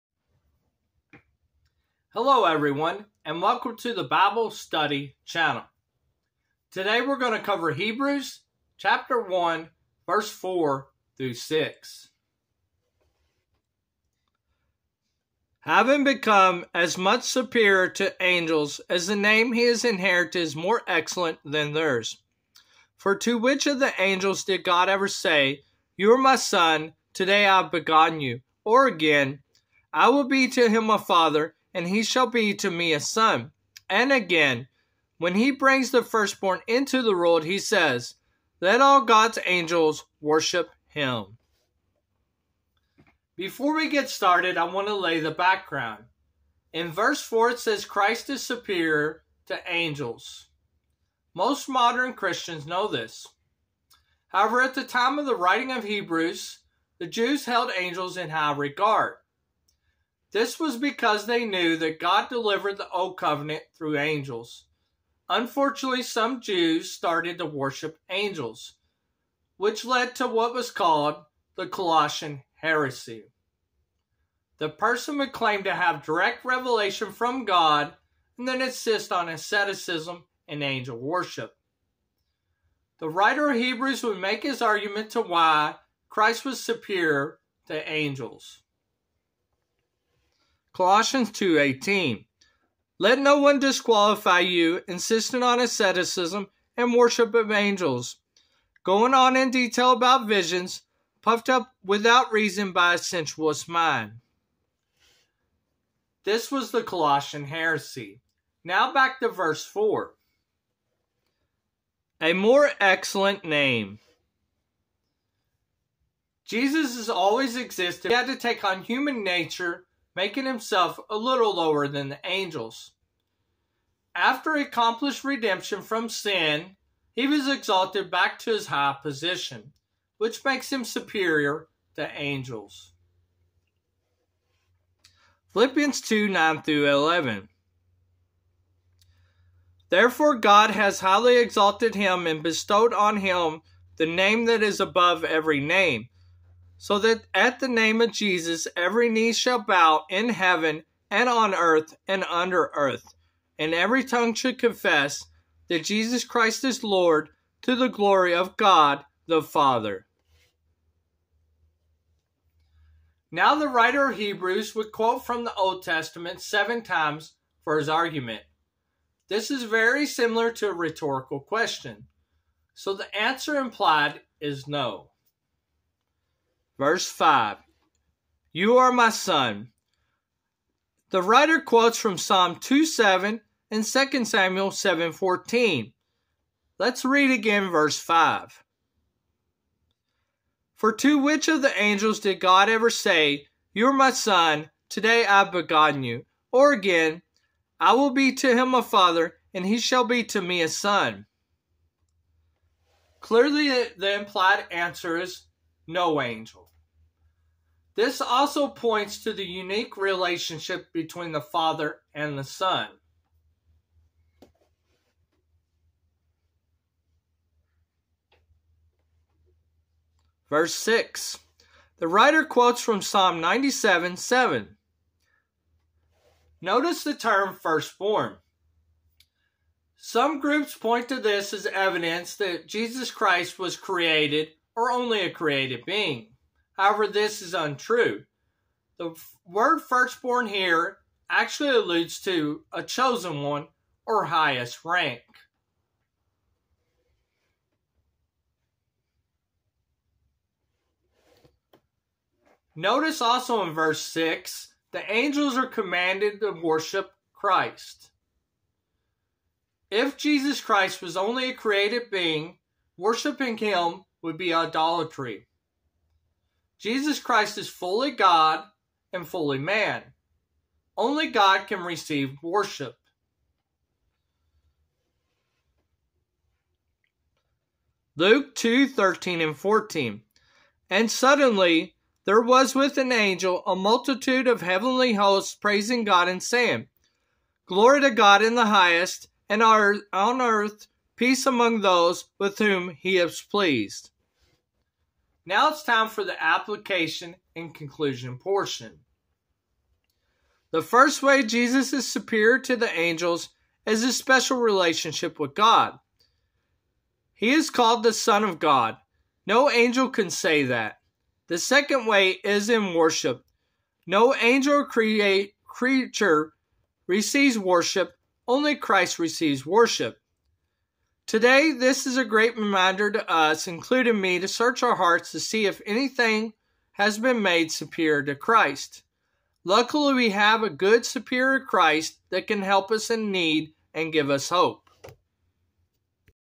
Igniting Curiosity: Why Christ is Superior to Angels!
In the book of Hebrews it says Christ is Superior to angels .  To most modern day Christians that's obvious.  However in the ancient church a hersey arose called the Colossian hersey that lead to angel worship . The writer of Hebrews would make his argument as to why Christ is Superior to angels .   We will also answer the question is Jesus just a created being is or is he fully man fully God .

The Superiority of Christ to angels hebrews The Colossian hersey